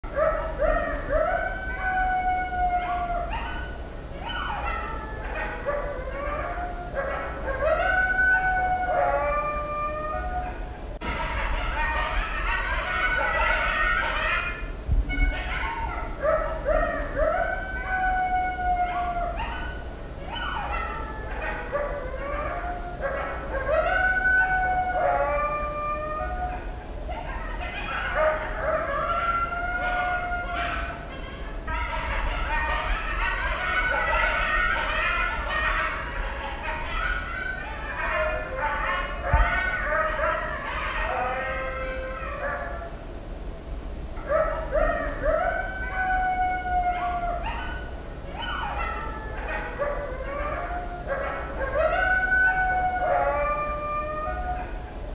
Coyote family howls and barks - Fr0ggy
recorded in the early hours of june 20 2024, PNW USA

free to use for any purposes

coyote pictured is in same family